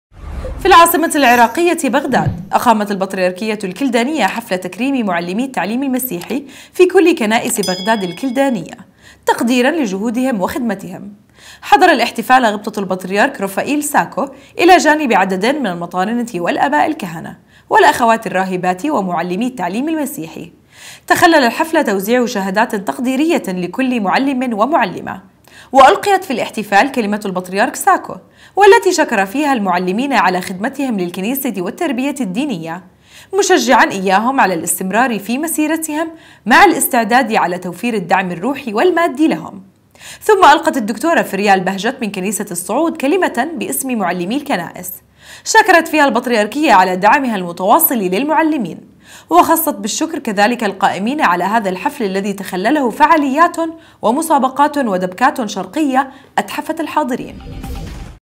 البطريرك ساكو يكرم معلمي التعليم المسيحي في بغداد
في العاصمةِ العراقيةِ بغداد أقامت البطريركيةُ الكلدانيه حفلَ تكريمِ معلمي التعليمِ المسيحي / في كلِ كنائس بغداد الكلدانيه تقديراً لجهودِهم وخدمتِهم / حضر الاحتفالَ غبطةُ البطريرك روفائيل ساكو / الى جانبِ عددٍ من المطارنةِ والاباءِ الكهنه / والاخواتِ الراهبات ومعلمي التعليمِ المسيحي / تخلل الحفلَ توزيعُ شهاداتٍ تقديريةٍ لكلِ معلمٍ ومعلمه / وألقيت في الاحتفال كلمةُ البطريرك ساكو / والتي شكر فيها المعلمينَ على خدمتِهم للكنيسةِ والتربيةِ الدينيه / مشجعاً إياهم على الاستمرارِ في مسيرتِهم / مع الاستعدادِ على توفيرِ الدعمِ الروحيِ والماديِ لهم / ثم القت الدكتورة فريال بهجت من كنيسةِ الصعود كلمةً باسمِ معلمي الكنائس / شكرت فيها البطريركيةَ على دعمِها المتواصلِ للمعلمين / وخصّت بالشكرِ كذلك القائمينَ على هذا الحفل الذي تخلله فعالياتٌ ومسابقاتٌ ودبكاتٌ شرقيةٌ أتحفت الحاضرين .